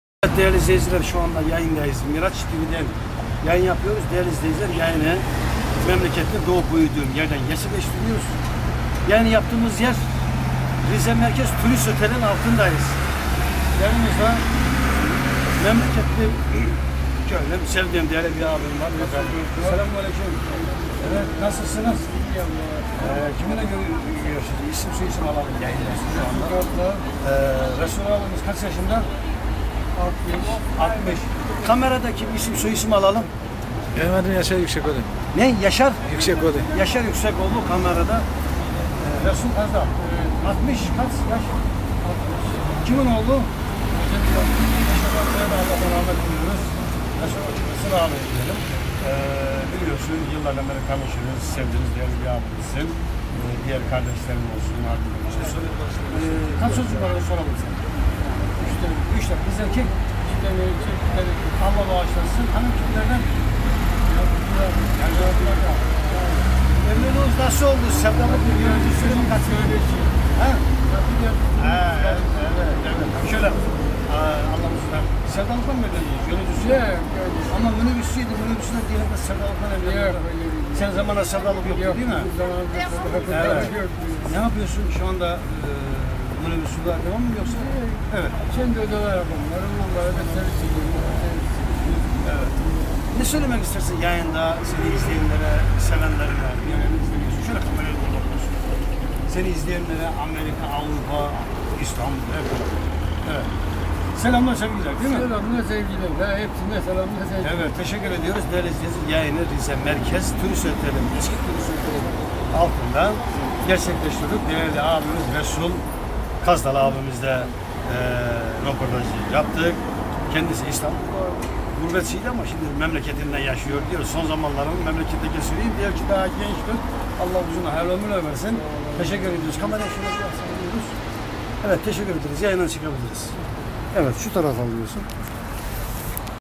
RİZE KÖMÜRCİLER. DOĞUMLU RESUL KAZDAL MİRAÇ TV. YE KONUŞTU
RİZE KÖMÜRCİLER. SİNGÖZ DOĞUMLU RESUL KAZDAL ABİMİZ MİRAÇ TV. YE KONUŞTU MİRAÇ TV. SİZİN SESİNİZ İYİ SEHİRLER DİLERİZ.